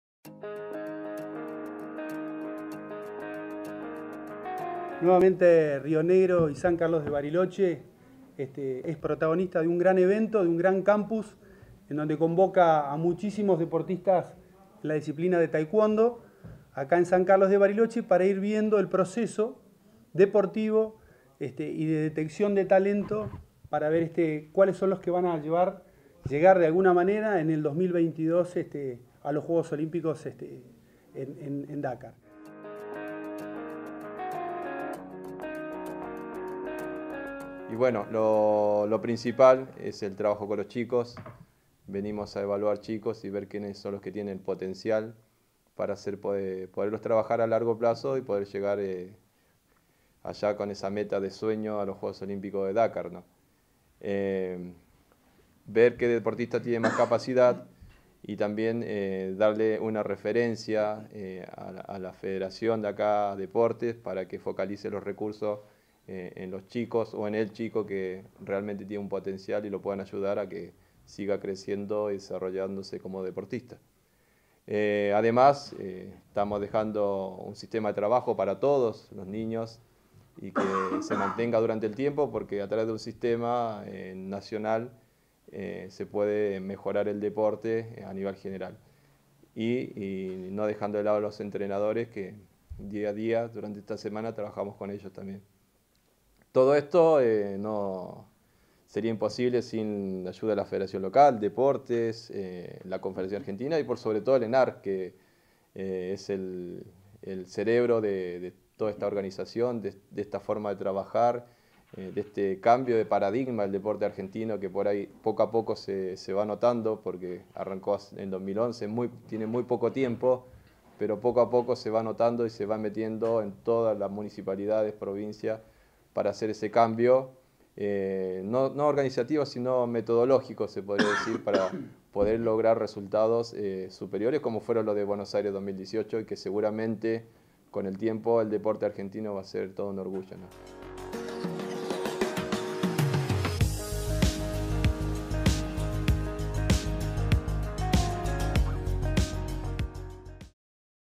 El Campus de Taekwondo toma fuerza con miras a los Juegos Olímpicos 2022
Jóvenes taekwondistas de todo el país participan del Campus Nacional de Taekwondo que se realiza por primera vez en Río Negro, y tiene sede en Bariloche hasta el 18 de mayo.

NOTA 1: Juan Pablo Muena, Subsecretario de Deportes Federados de Río Negro

NOTA 2: Jeovanni Baeza, Ccoordinador y Metodólogo de la Confederación Argentina de Taekwondo